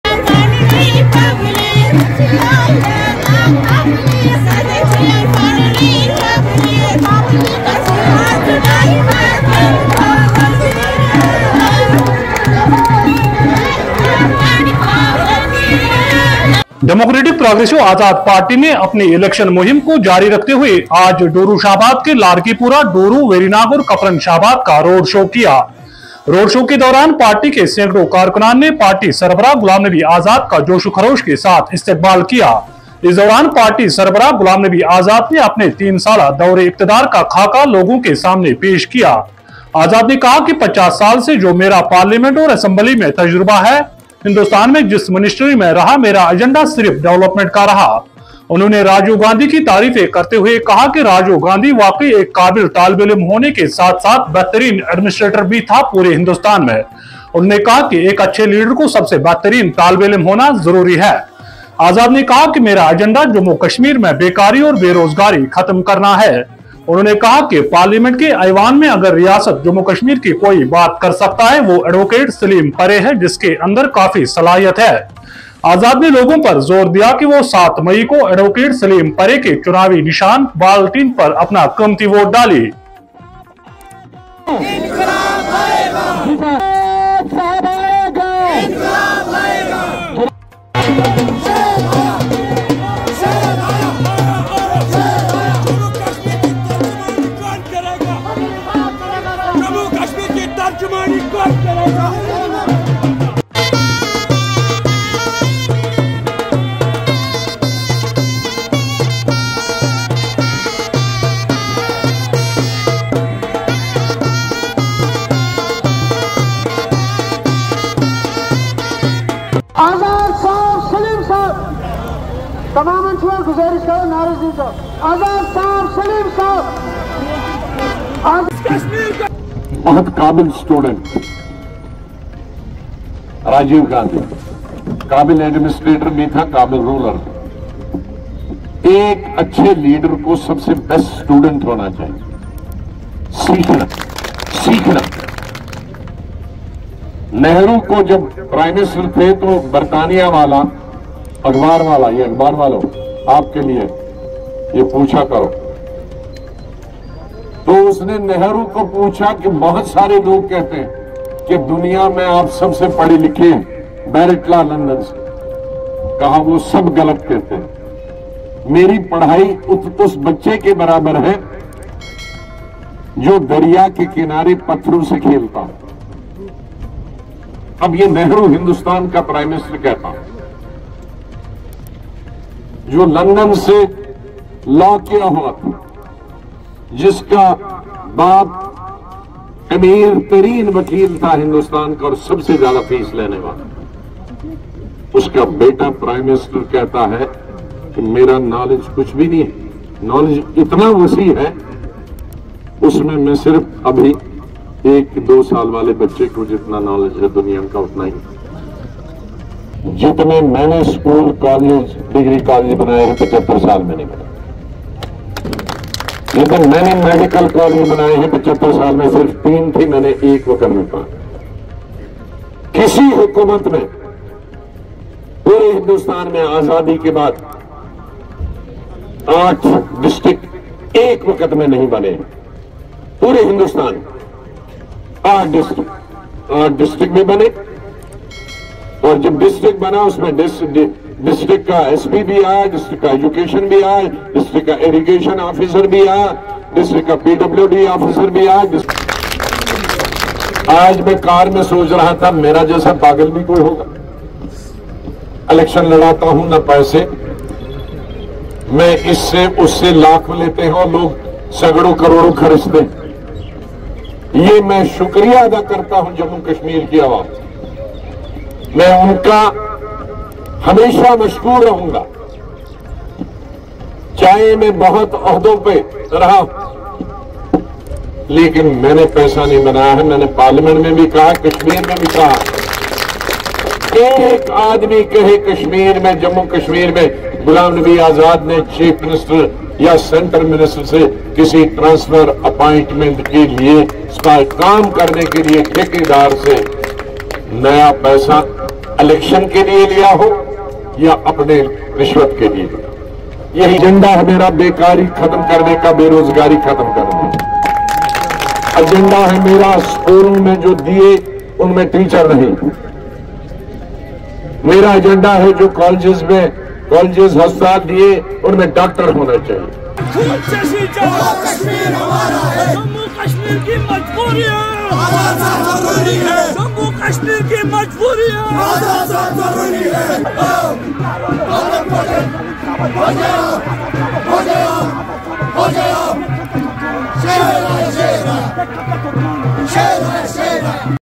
DPAP Road Show At Dooru Anantnag
DPAP, continuing its election campaign, today conducted a road show in Larkipura, Dooru, Verinag and Kaparn Shahabad of Dooru constituency.